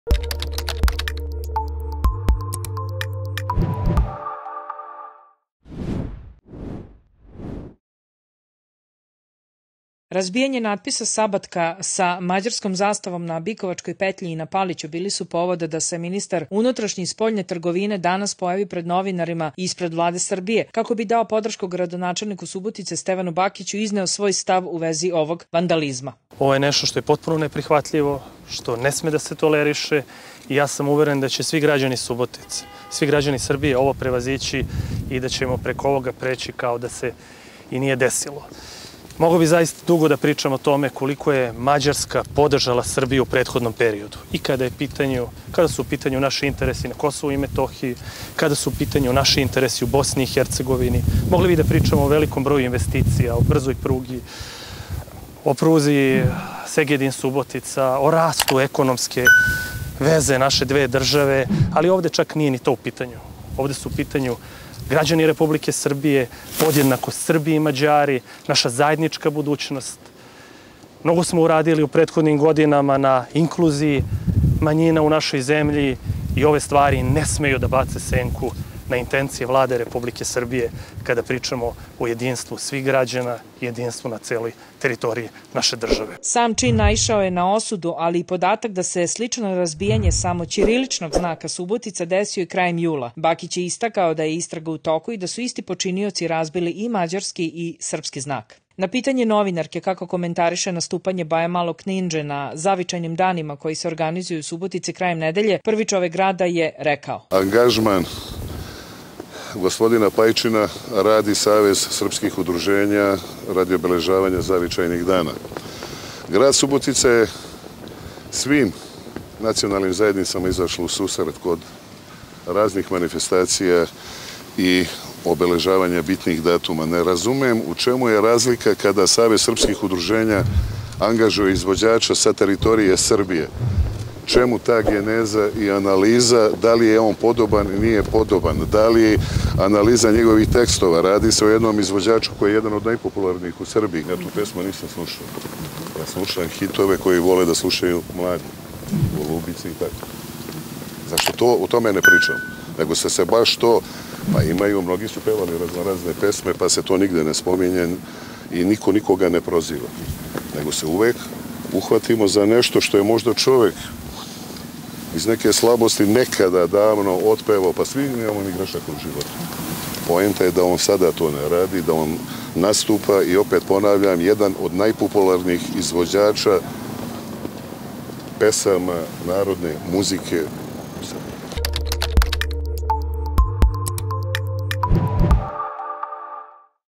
"Szabadka" ili "Суботица", povod je za razbijanje
Razbijanje natpisa „Szabadka“ sa mađarskom zastavom na Bikovačkoj petlji i na Paliću, bili su povod da se ministar unutrašnje i spoljne trgovine danas pojavi pred novinarima, ispred Vlade Srbije kako bi dao podršku gradonačelniku Subotice Stevanu Bakiću i izneo svoj stav u vezi ovog vandalizma.
Sam čin naišao je na osudu, ali i podatak da se slično razbijanje, samo ćiriličnog znaka „Subotica“ desio i krajem jula. Bakić je istakao da je istraga u toku, i da su isti počinioci razbili i mađarski i srpski znak.
Na pitanje novinarke kako komentariše nastupanje  Baje Malog Knindže na Zavičajnim danima koji se organizuju u Subotici krajem nedelje, prvi čovek grada je rekao.